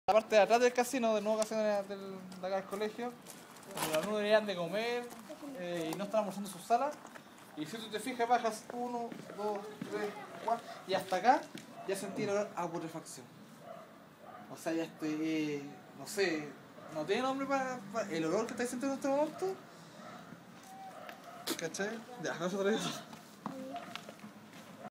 Petitorio interno Chilean Eagles College (21/22)
Un estudiante y un apoderado del Chilean Eagles College muestran su liceo y explican parte de su petitorio interno. La Florida, Santiago, Chile